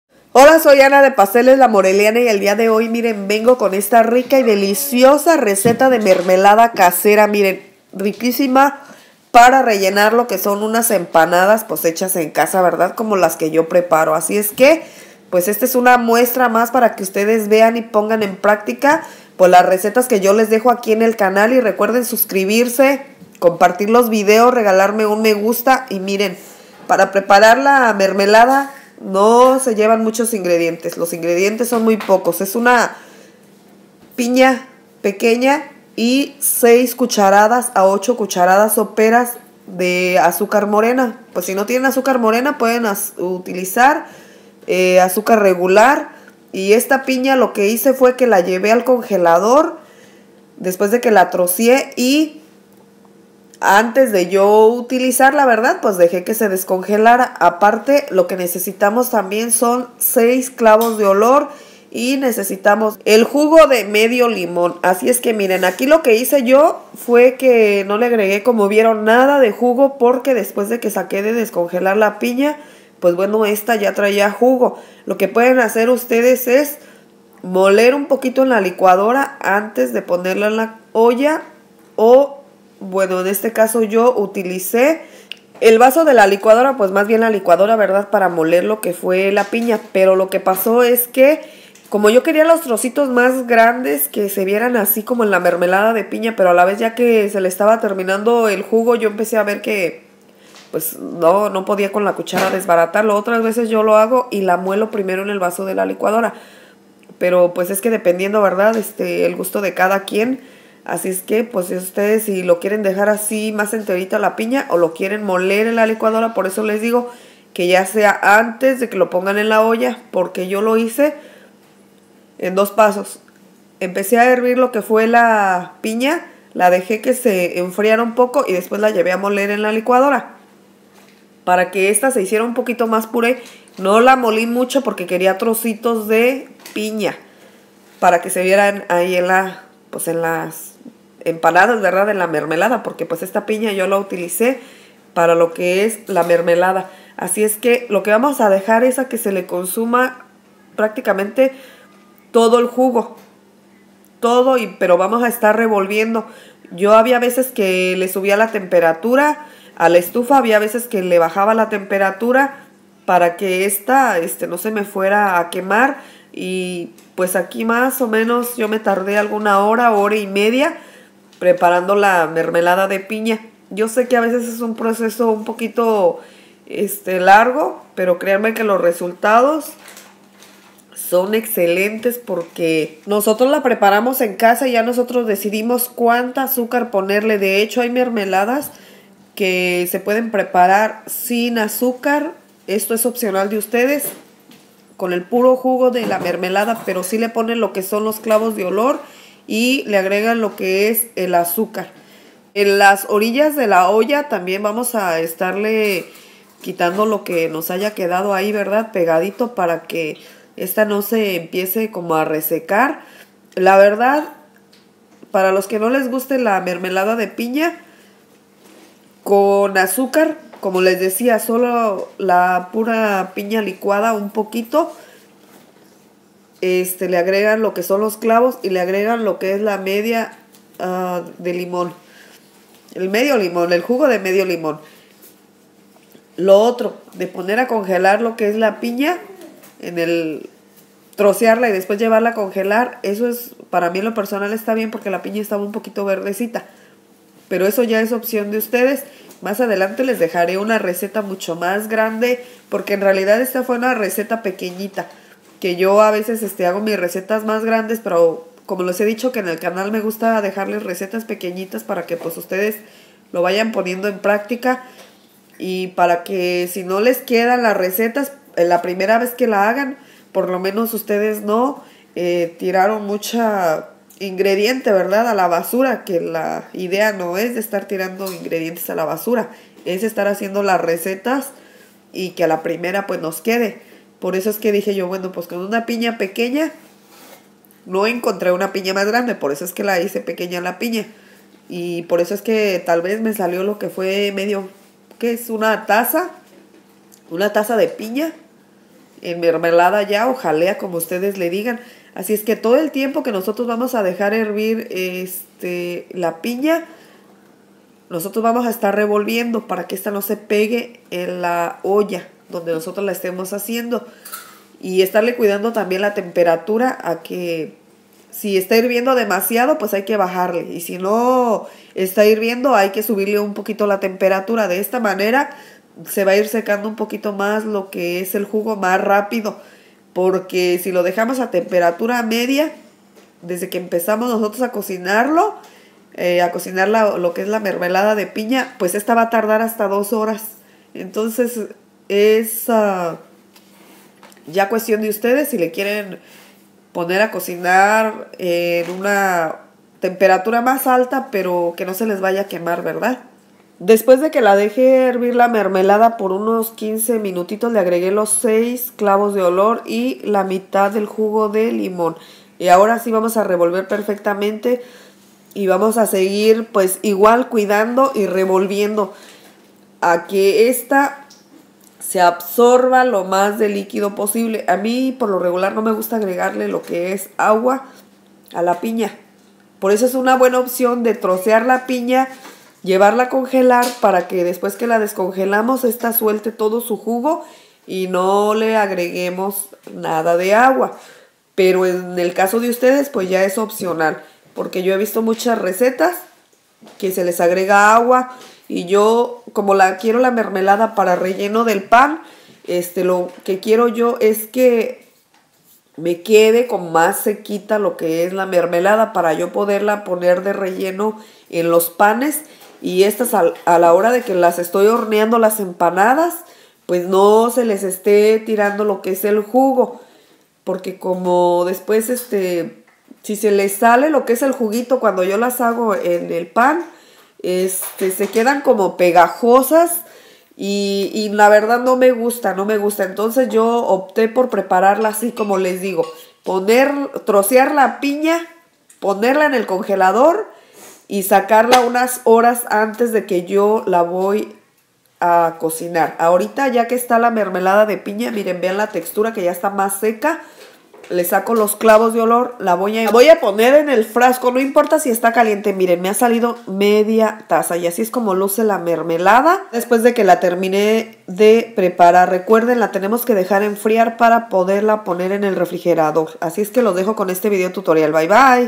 MERMELADA DE PIÑA 🍍 O JALEA DE PIÑA 🍍 FACIL DE PREPARAR PARA RELLENO DE PAN
RECETA DEL PAN MEXICANO CONCHAS CASERAS

CON ESTA RECETA DE PAN DE CONCHAS PUEDES PREPARAR LAS EMPANADAS QUEDAN RIQUISIMAS TE LA RECOMIENDO Y SOBRE TODO ES MUY FACIL DE PREPARAR
LA MASA Y LA JALEA O MERMELADA

INGREDIENTES
1 pina chica o grande
6 a 8 cucharadas soperas de azucar morena o regular
6 clavos de olor
la mitad del jugo de un limon